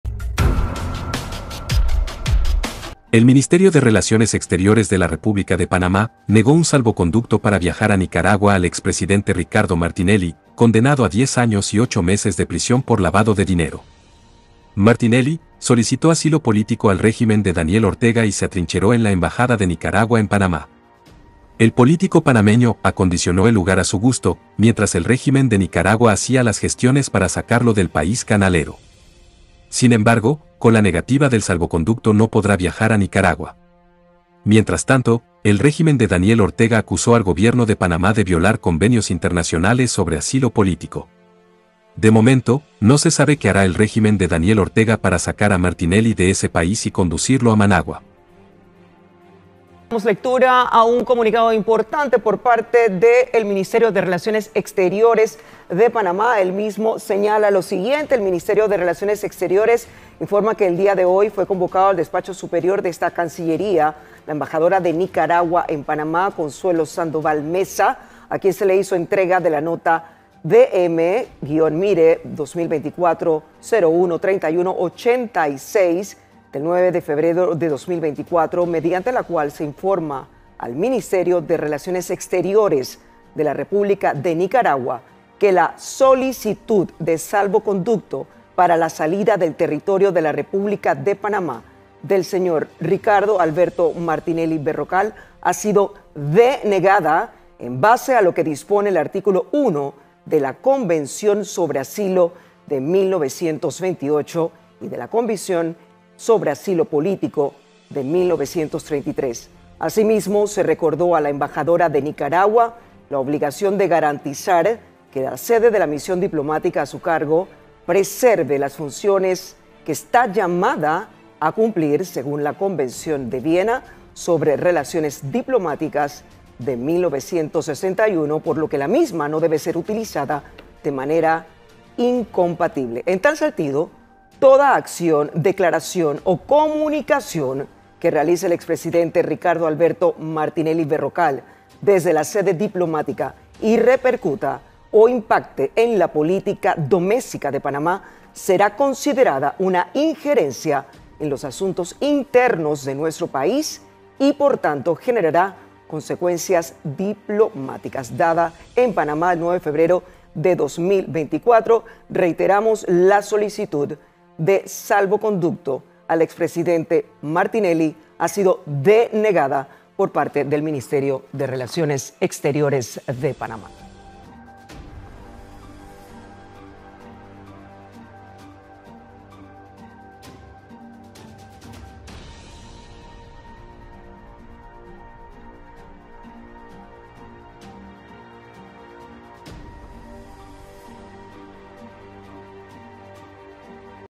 MALAS NOTICIAS para Ricardo Martinelli: Niegan salvoconducto y no podrá viajar a Nicaragua
Malas noticias para Ricardo Martinelli, el Ministerio de Relaciones Exteriores negó SALVOCONDUCTO por lo que no podrá viajar a Nicaragua.